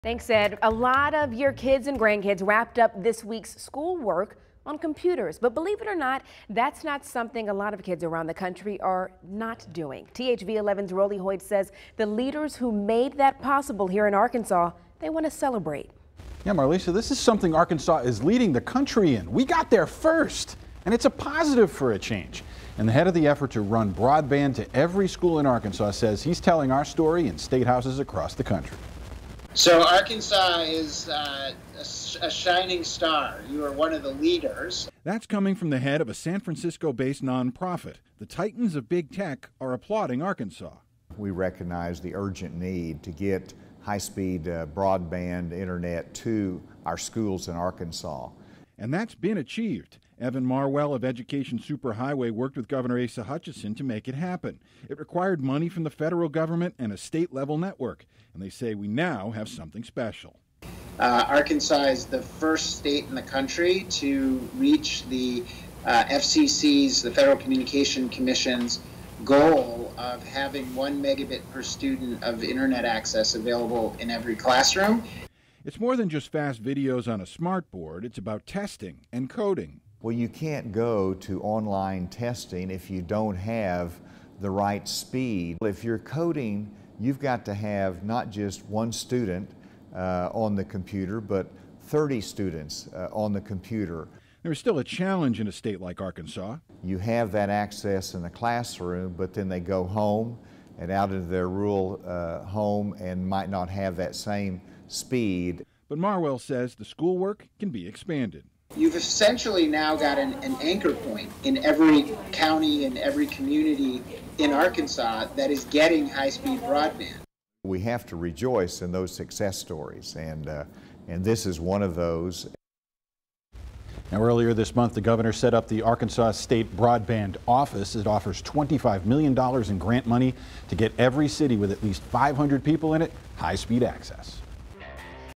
Arkansas becomes nationwide leader in connecting schools to internet
Arkansas is the first state in the country to reach the F.C.C.'s goal of having one megabit per student in every classroom in the state.